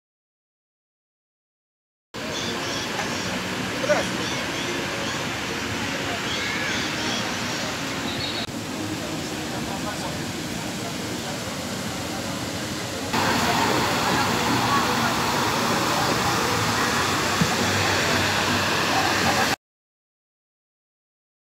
В Одессу пришло лето! (19 июня 2021)
Одесса, 19 июня 2021 г. В Одессе жарко и солнечно.